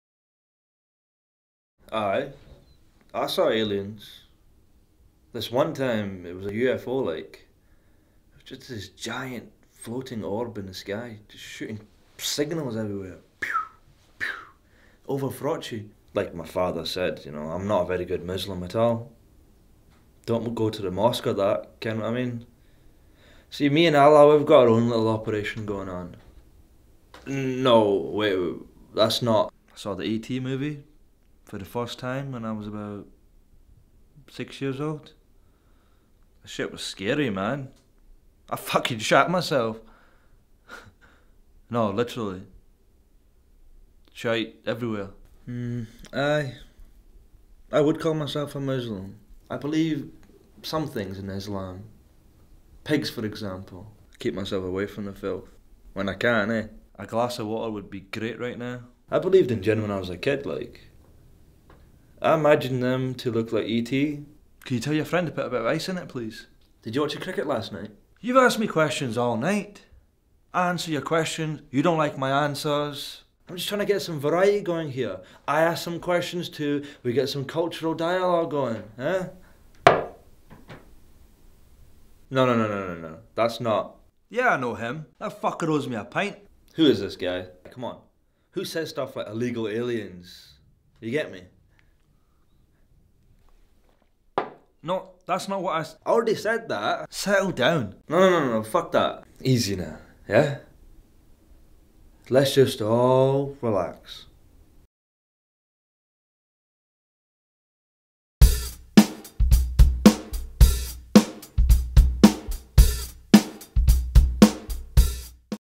Lifted Trailer Free Fringe 2015 (18)
This Edinburgh Fringe
Triad Pictures Presents

- Lifted -

*Strong language*

Directed by Deborah Sasha Hahn
Written by Sara Shaarawi & Henry Bell
Performed by Ikram Gilani

@ The Cellar Monkey, Edinburgh
6th-20th August 2015 14:30-15:30

This trailer was produced by Raptor Filmz Ltd.